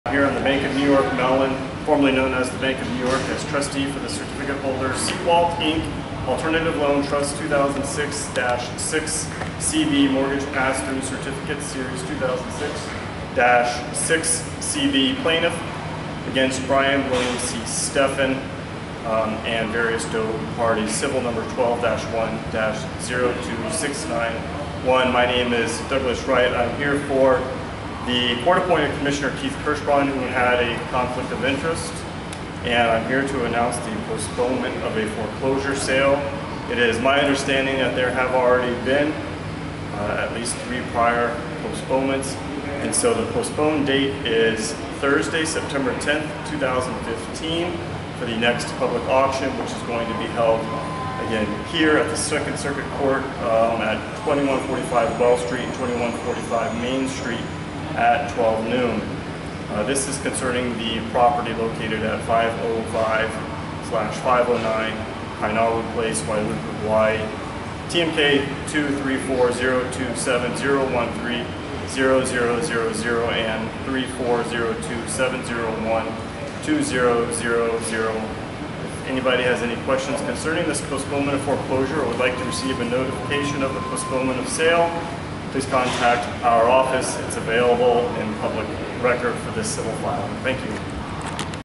Foreclosure Auction Maui Hawaii 8/4/2015 postponement of 505 509 Kainalu PL
Foreclosure Auction in the county of Maui Hawaii at 2145 Main Street Wailuku, Hi. 96793 Keith D. Kirschbraun The following property is facing foreclosure by their respective lien holder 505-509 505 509 Kainalu PL Wailuku, HI 96793